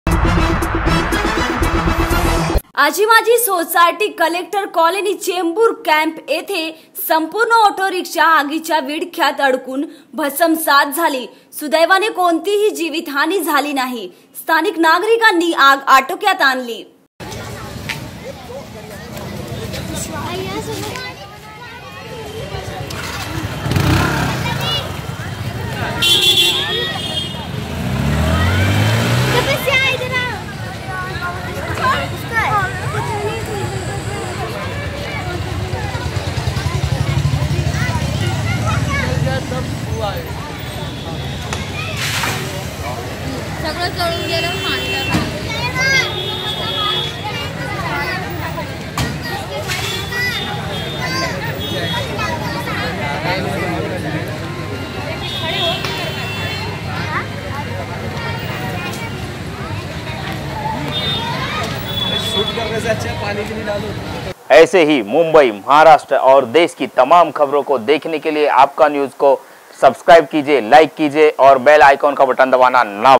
चेम्बूर आजी माजी सोसायटी कलेक्टर कोलनी चेंबुर येथे ओटो रीक्षात लागली आग || Aapka News
आजी माजी सोसायटी कलेक्टर कोलनी चेंबुर येथे ओटो रीक्षात लागली आग
स्थानिक नागरिकांनी आटोक्यात आणली आग